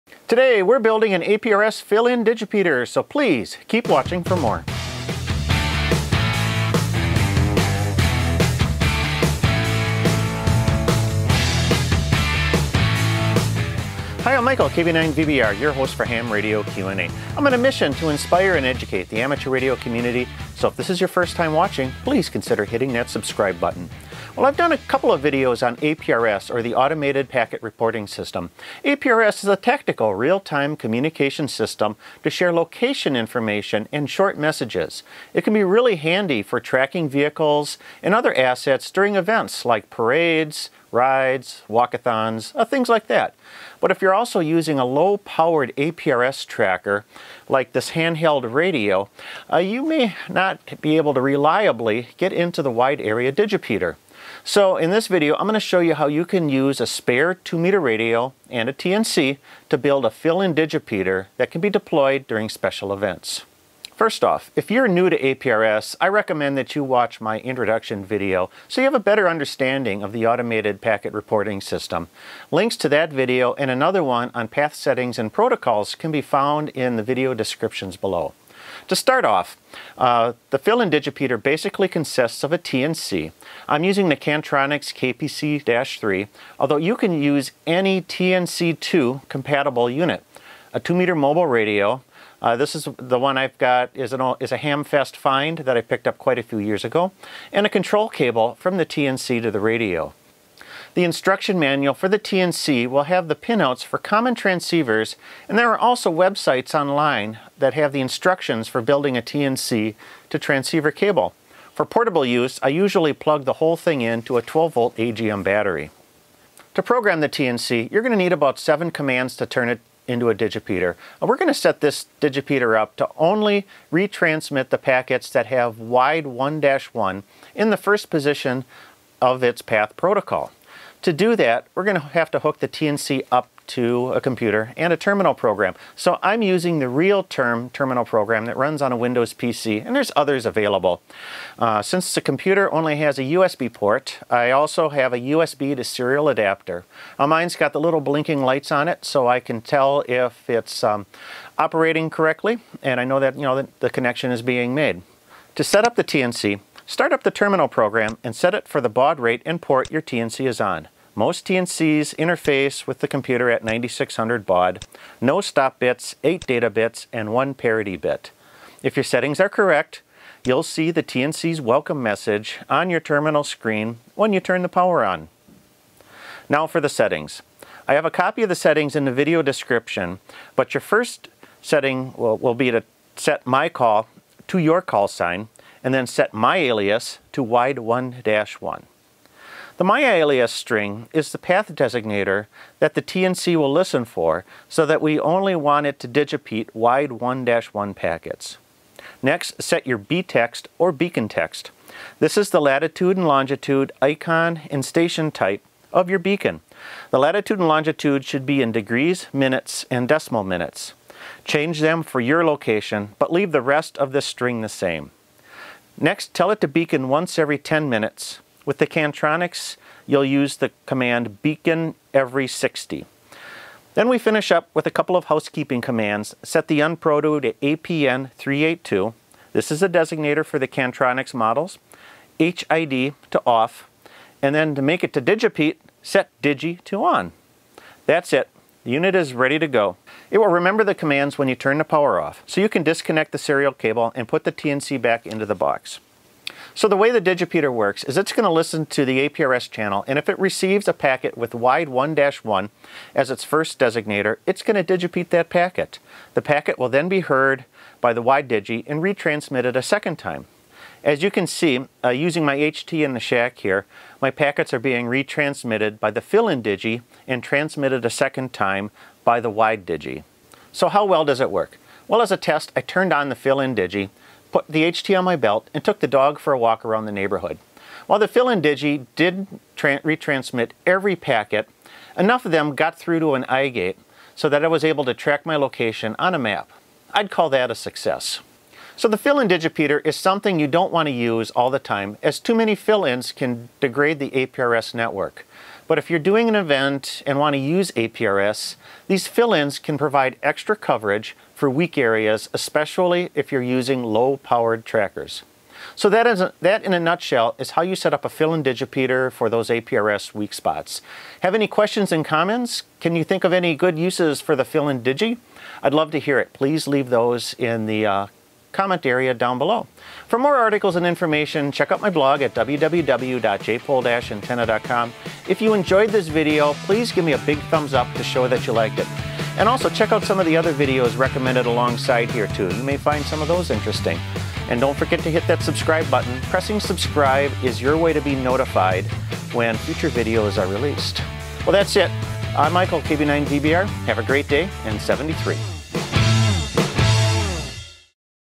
Build an APRS Fill In Digipeater - Ham Radio Q&A
APRS or the Automated Packet Reporting System can be really handy for tracking vehicles and other assets during events like parades, rides or walkathons. But if you are using a low powered APRS tracker, like a handheld radio, you may not be able to reliably get into the wide area digipeater. In this video I’m going to show you how you can use a  spare 2 meter radio and TNC to build a fill in digipeater that can be deployed during special events.

Products Mentioned:

USB Serial Adapter with LED Indicators

My Wide1-1 Fill In Digipeater Settings:

MYCALL KB9VBR-1
MYALIAS Wide1-1
BTEXT !DDMM.mmN1DDDMM.mmW#W1-1 Fill In Digi
BEACON E 60
UNPROTO APN382
HID OFF
DIGI ON

Colophon:
Camera - Panasonic GH4
Lens - Panasonic 12-35 f2.8
Microphone: Azden Pro-XD wireless lav